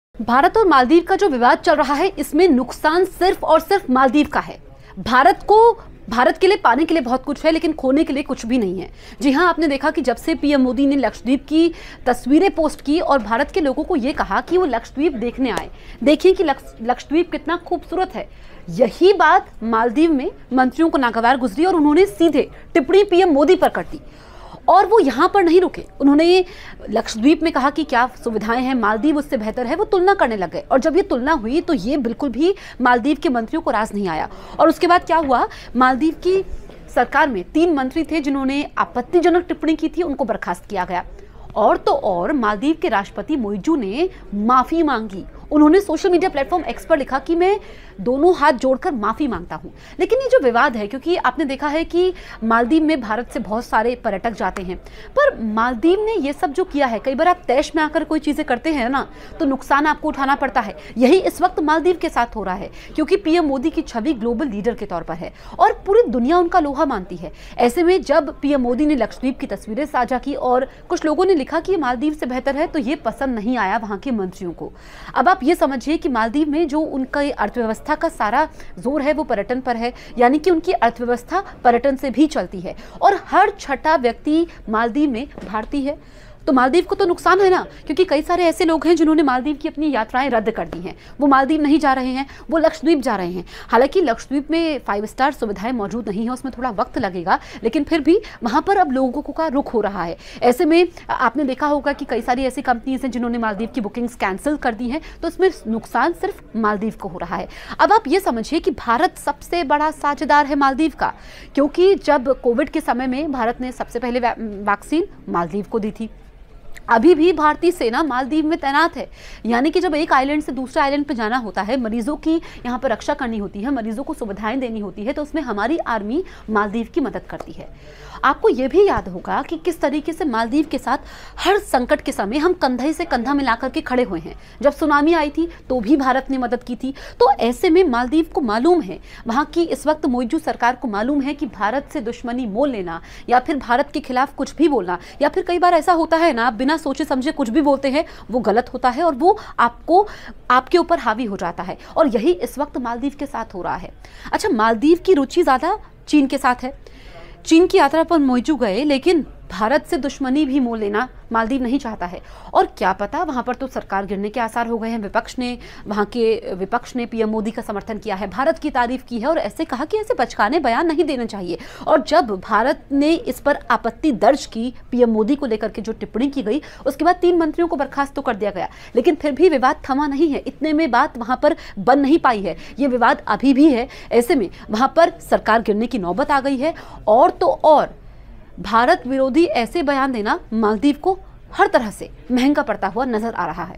India Maldives Row :क्यों शुरू हुआ मालदीव और लक्षद्वीप का झमेला,असल वजह आई सामने|Lakshadweep|PM Modi
मालदीव और लक्षद्वीप का झमेला चल रहा है। प्रधानमंत्री नरेंद्र मोदी ने अपील की है कि लोगों को लक्षद्वीप की सैर ज़रूर करनी चाहिए। मालदीव वाले बुरा मान गए। उन्होंने भारत और भारतीय प्रधानमंत्री के खिलाफ टिप्पणी शुरू कर दी। भारत ने इस पर कड़ी आपत्ति जताई। आख़िर मालदीव को तीन मंत्रियों को निलंबित कर दिया गया।

There is a problem going on between Maldives and Lakshadweep. Prime Minister Narendra Modi has appealed that people should visit Lakshadweep. The people of Maldives felt bad. He started commenting against India and the Indian Prime Minister. India expressed strong objection to this. Eventually three ministers of Maldives were suspended.